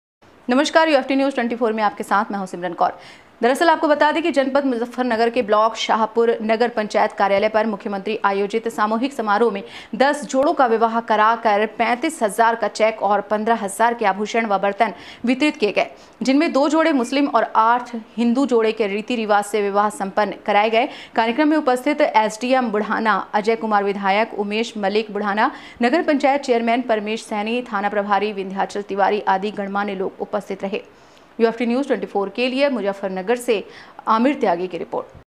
UFT NEWS#24#शाहपुर नगर पंचायत कार्यालय पर आयोजित सामूहिक विवाह समारोह कराया गया
शाहपुर नगर पंचायत कार्यालय पर आयोजित सामूहिक विवाह समारोह कराया गया

दरअसल आपको बता दें कि  जनपद मुजफ्फरनगर के ब्लॉक शाहपुर नगर पंचायत कार्यालय पर मुख्यमंत्री आयोजित सामूहिक समारोह में 10 जोड़ों का विवाह करा कर 35,000 का चेक और 15000 के आभूषण व बर्तन वितरित किए गए जिनमें दो जोड़े मुस्लिम और 8 हिंदू जोड़े के रीति रिवाज से विवाह संपन्न कराया गया कार्यक्रम में उपस्थित एसडीएम बुढाना अजय कुमार विधायक उमेश मलिक बुढ़ाना नगर पंचायत चेयरमैन परमेश सैनी थाना प्रभारी विंध्याचल तिवारी आदि गणमान्य लोग उपस्थित रहे ।

मुजफ्फरनगर से ब्यूरो रिपोर्ट आमिर त्यागी
हमसे जुडने के लिए इन लिंक्स पर फॉलो करें: